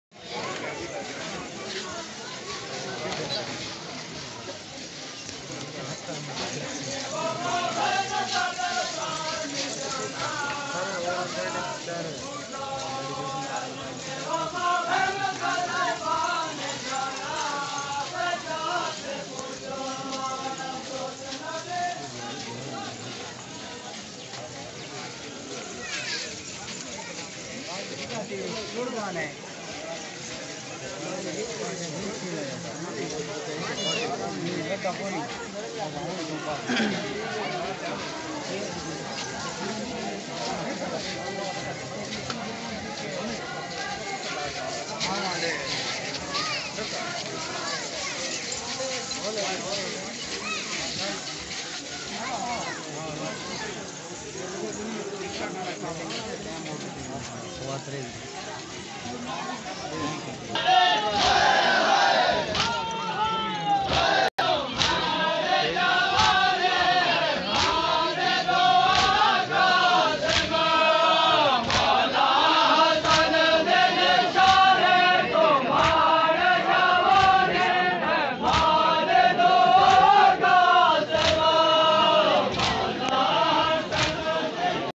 the most hard and diffical process of taziyadari of kath gar azadari.it is unbelievable work we done
kath garh azadari ka sb se mushkil tareen kaam  likin momneen ki muhabat k  agay kuch b ni hy.